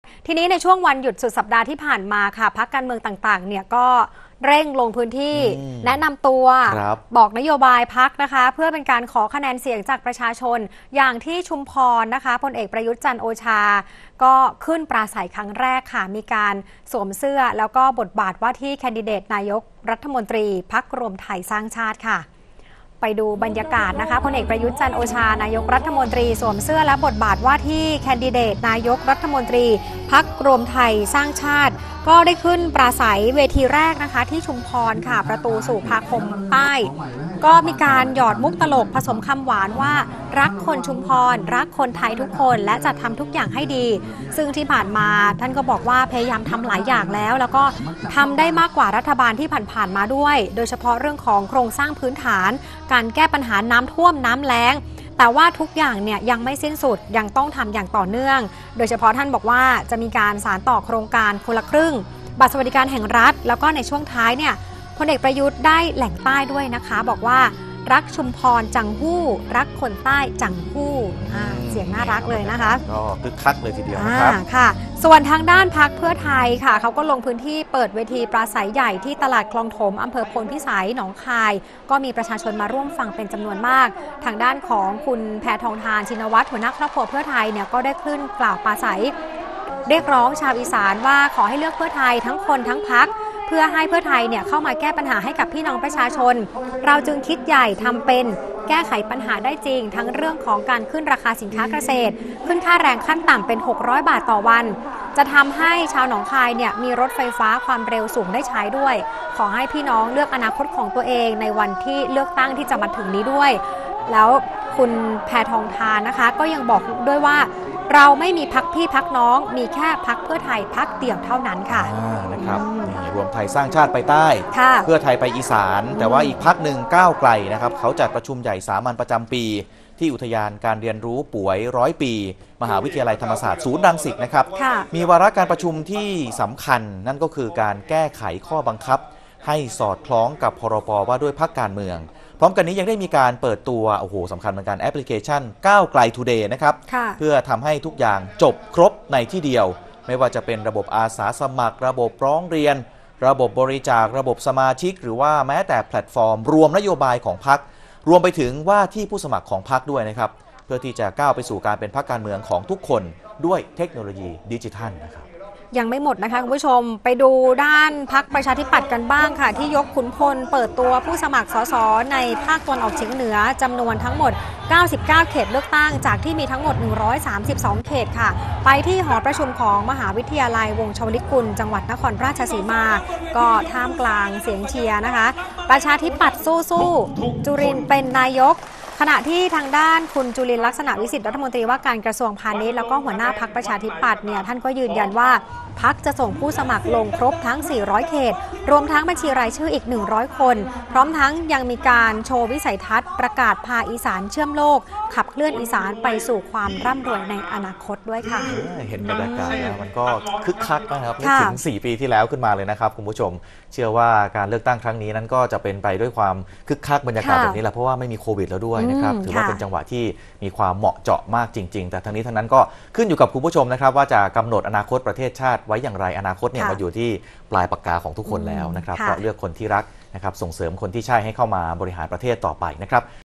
พล.อ.ประยุทธ์ ปราศรัยใหญ่ครั้งแรกในนามสมาชิก พรรครวมไทยสร้างชาติ ข่าวดึก วันที่ 28 มกราคม 2566
ในช่วงวันหยุดสุดสัปดาห์นี้ พรรคการเมืองต่างๆ เร่งลงพื้นที่เพื่อขอคะแนนเสียงจากประชาชน อย่างที่จังวัดชุมพร พลเอกประยุทธ์ จันทร์โอชา ขึ้นปราศรัยครั้งแรก สวมเสื้อและบทบาทว่าที่แคนดิเดตนายกรัฐมนตรี พรรครวมไทยสร้างชาติ